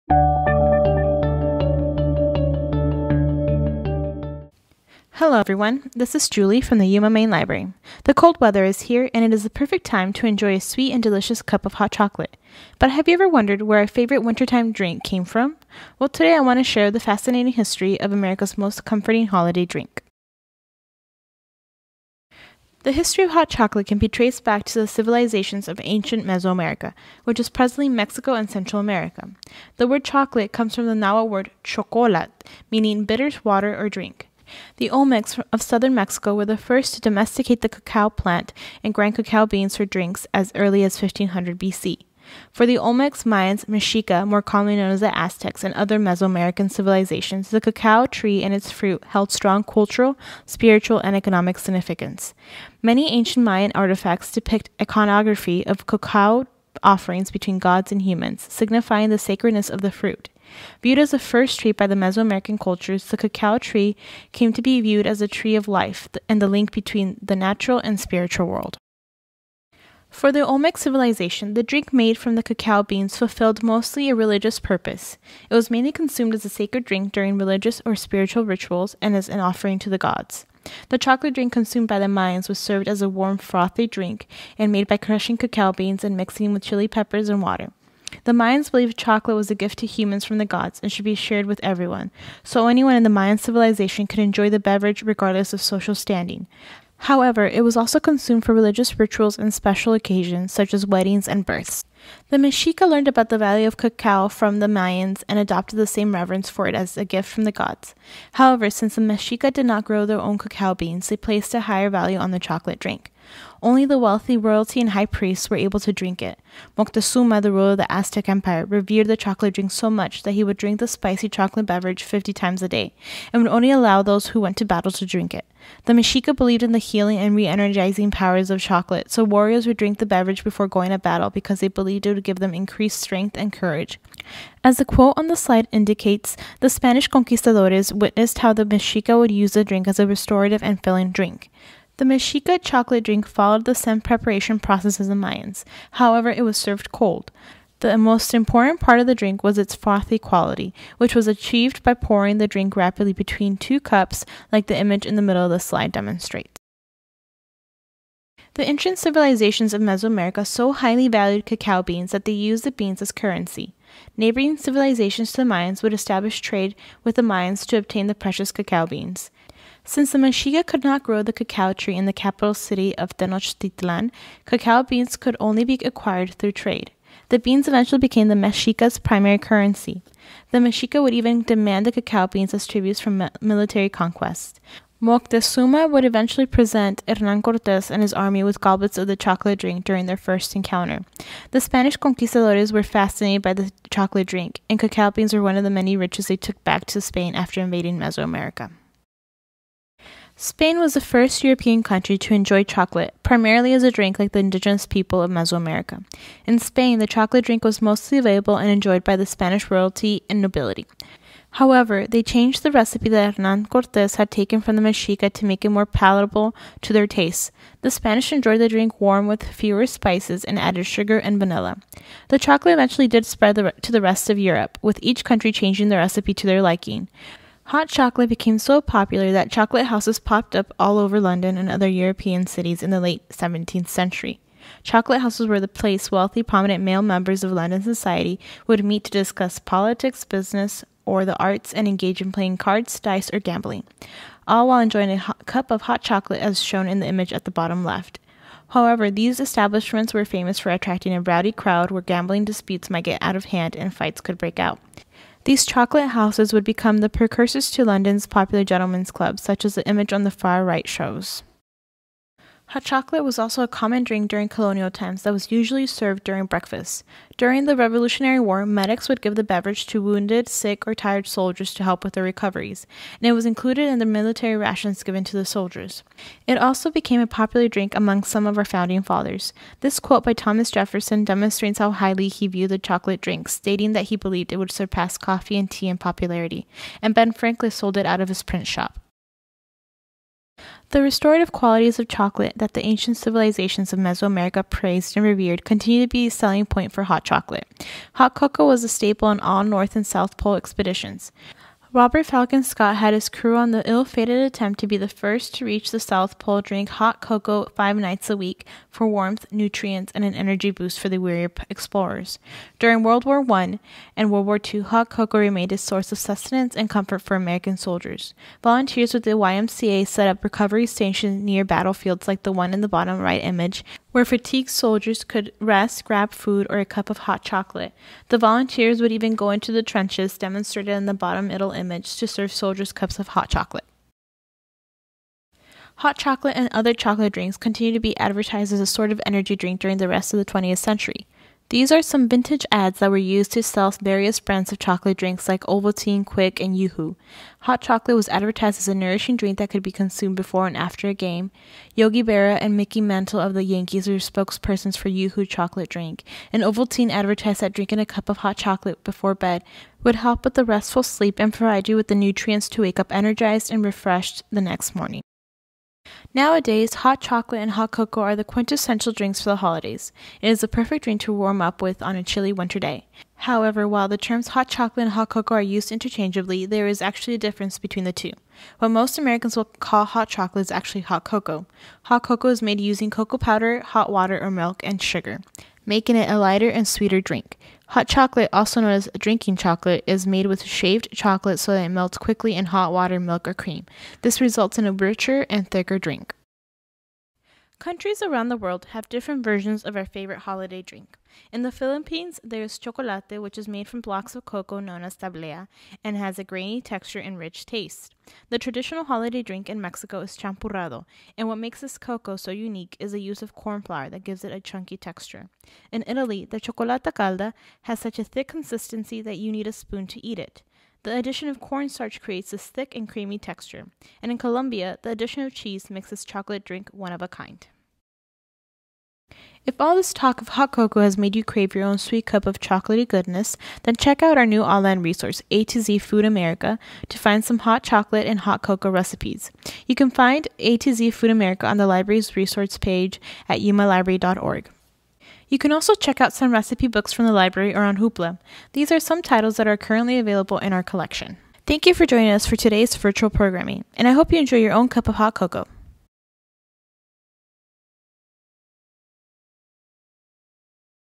The History of Hot Chocolate
YCLD Librarian Julie presents the delicious origins of hot chocolate.